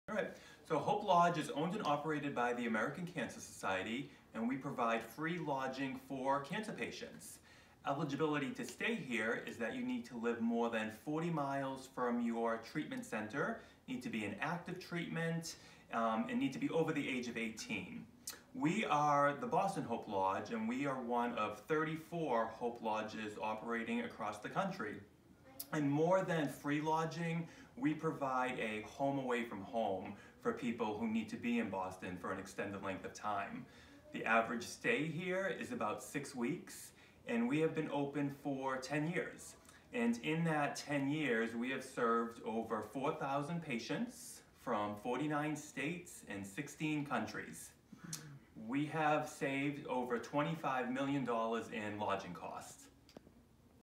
Matt Blom-Manager of the Hope Lodge in Boston, describes the Facility
Our fundraising luncheon for the Hope Lodge in Boston in on June 2, 2019